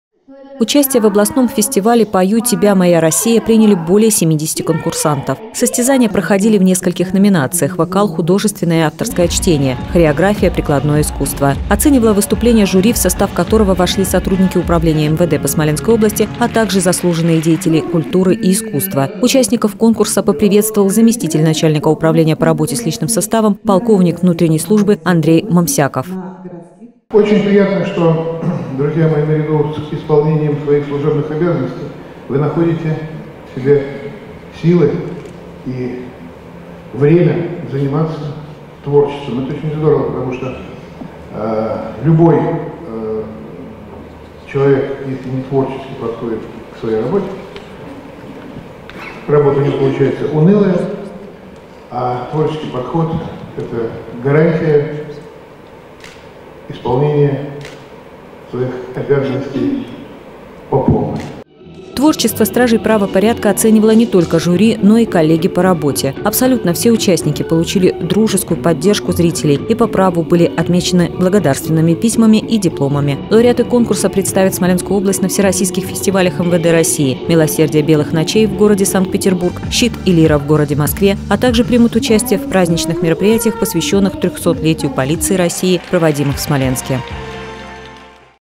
Десна-ТВ: Музыкальной творческий конкурс среди полицейских
В культурном центре управления УМВД России по Смоленской области прошел региональный этап Всероссийского фестиваля музыкального творчества МВД России «Щит и Лира». Сотрудники органов внутренних дел, а также члены их семей, принявшие участие в конкурсе, подарили зрителям настоящий праздник.